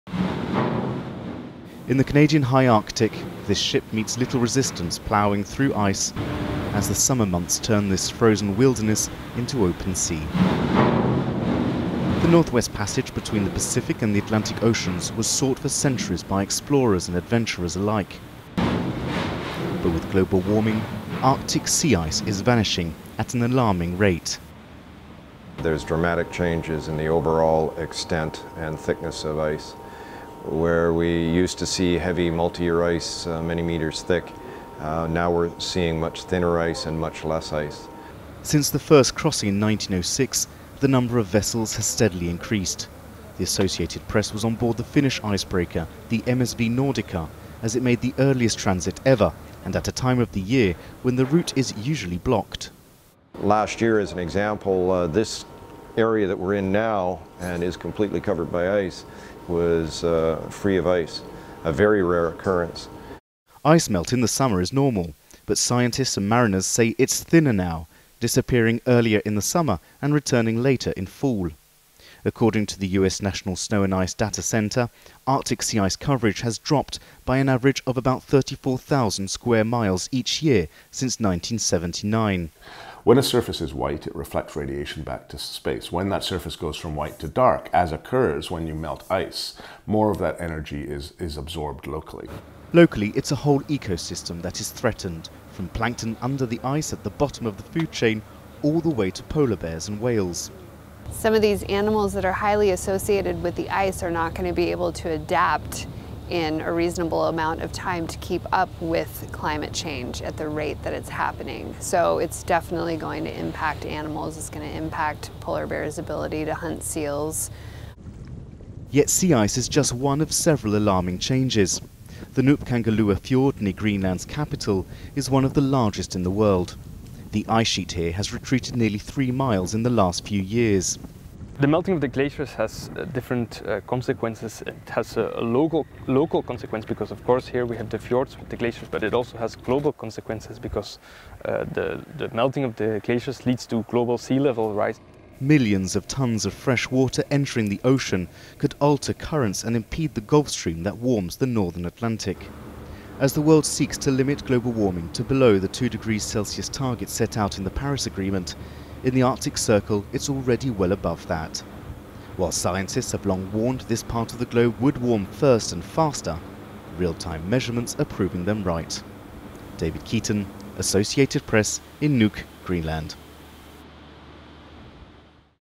Arctic Sea Ice is Disappearing at Alarming Rate
Arctic sea ice melt in the summer is normal but experts say it's thinner now, disappearing earlier and returning later in the fall. Since the first orbital images in 1979, sea ice coverage dropped an average of 34,000 square miles a year. (Aug. 14)